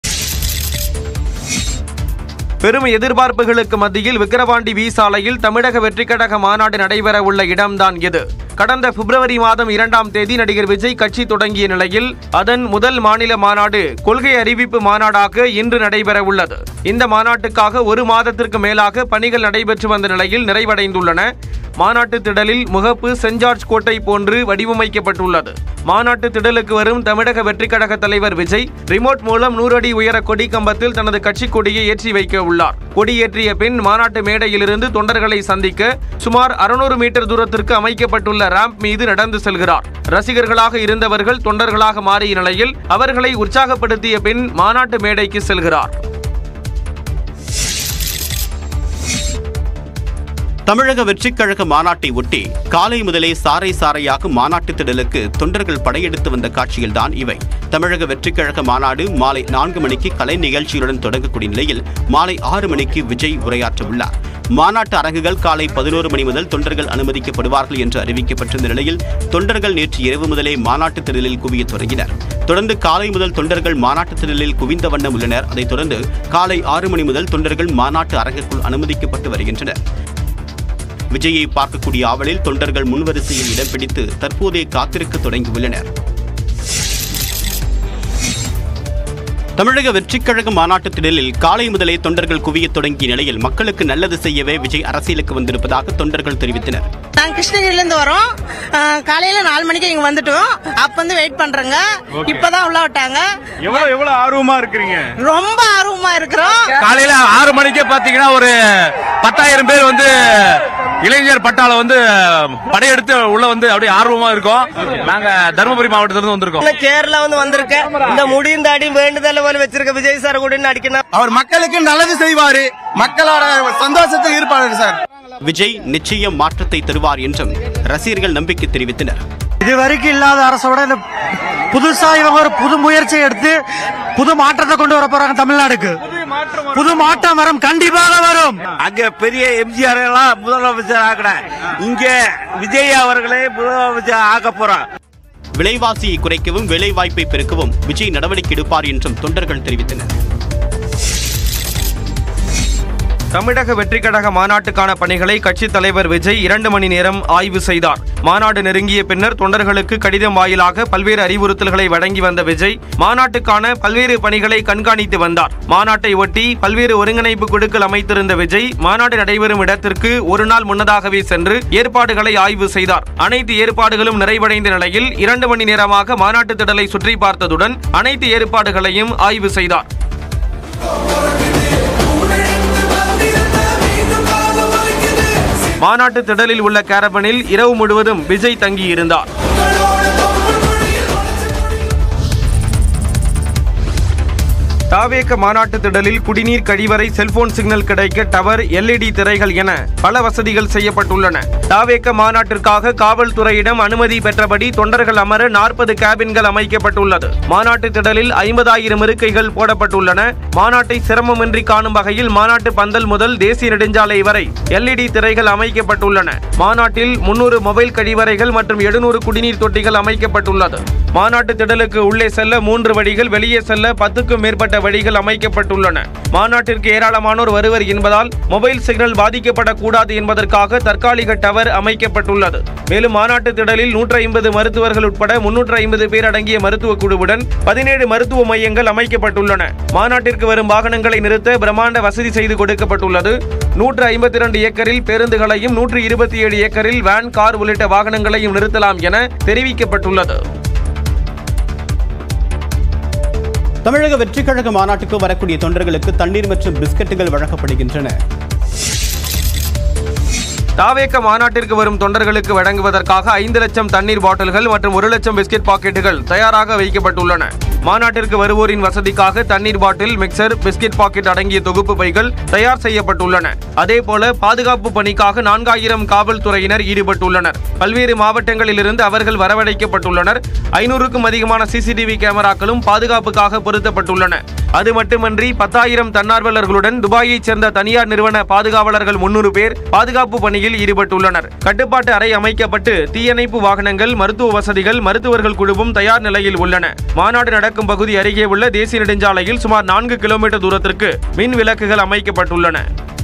TVK Maanadu | மாநாட்டு மேடைக்கு விஜய் எப்போது வருவார் தெரியுமா? | Vijay Maanadu | Thalapathy Vijay
இன்று மாலை 4 மணிக்கு மாநாட்டை தொடங்க திட்டம் - செயின்ட் ஜார்ஜ் கோட்டை போல மாநாட்டு திடலின் முகப்பு - 100 அடி உயர கொடிக்கம்பத்தில் கொடியேற்றுகிறார் விஜய்  | Vijay Maanadu | Thalapathy Vijay | TVK Maanadu

list=PLZjYaGp8v2I8Jl8PfHo2MQdWKs42-15cX